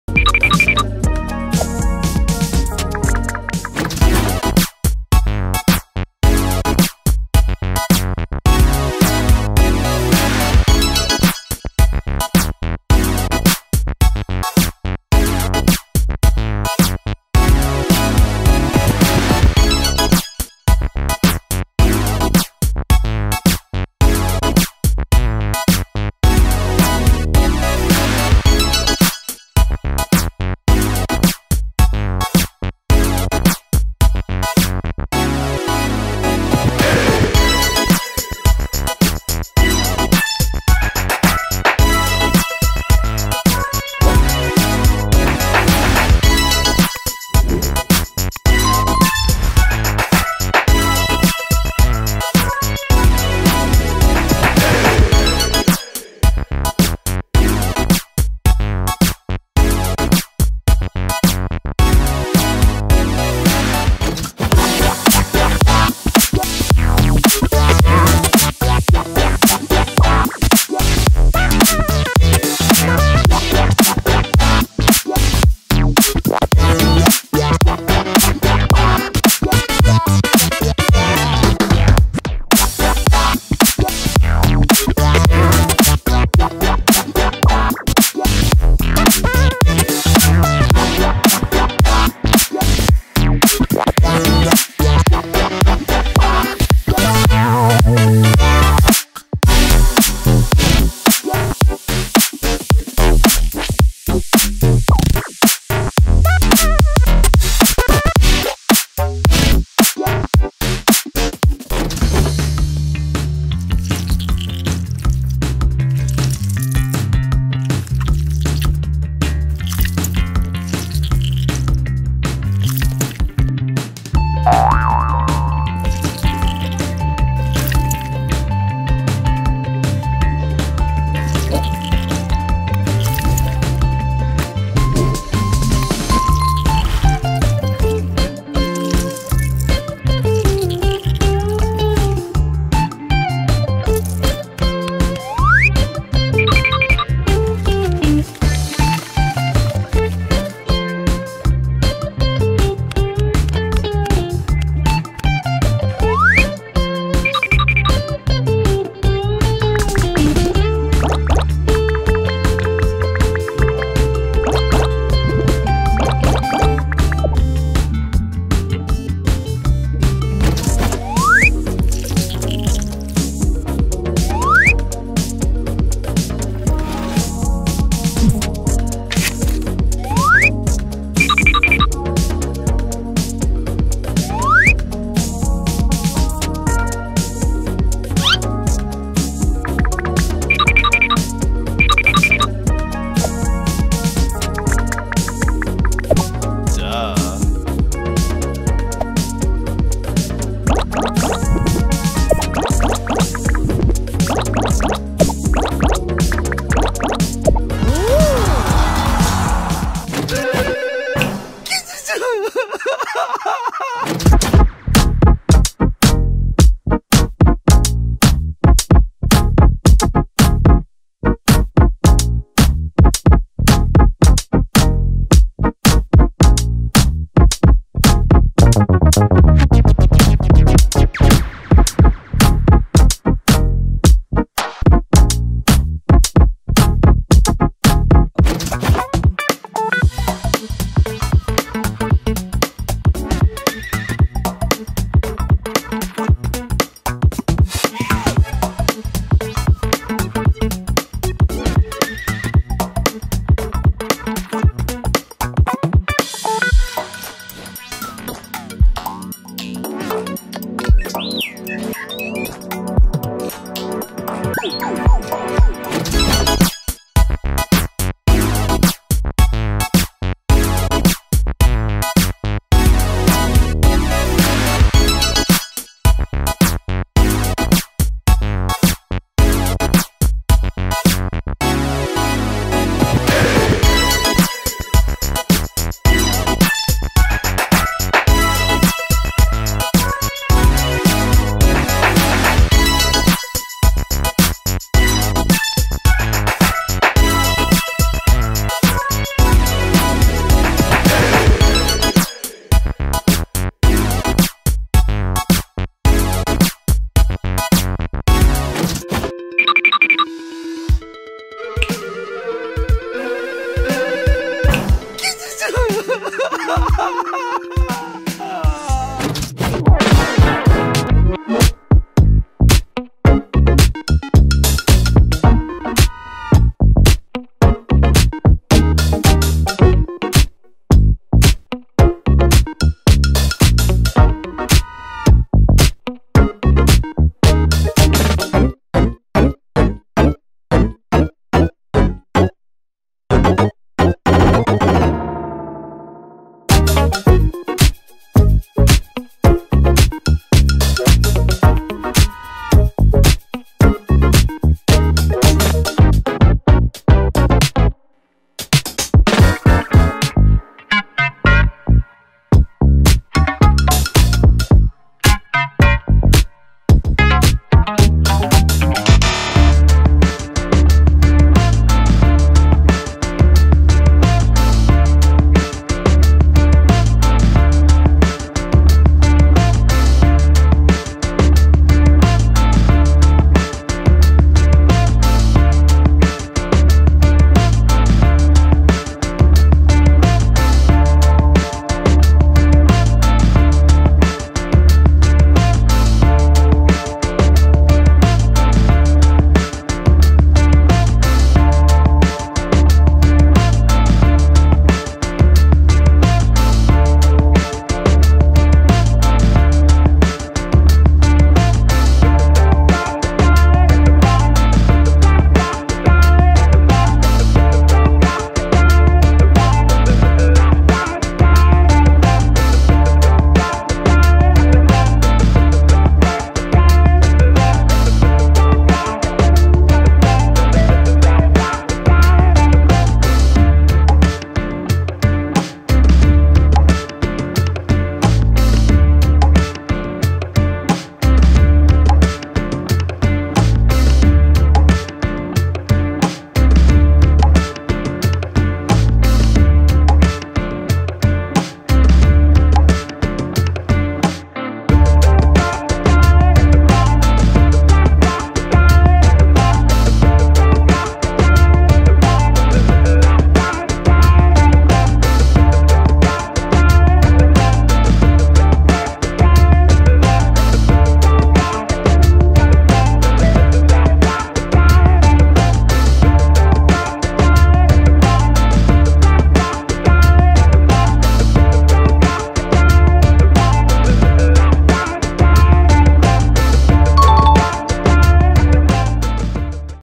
Drawing Compilation: Abstracted Version of Kaufmo, Gangle & Caine from The Amazing Digital Circus
Today U Will See How I Drawing Compilation with Abstracted Version of Kaufmo Gangle & Caine from The Amazing Digital Circus!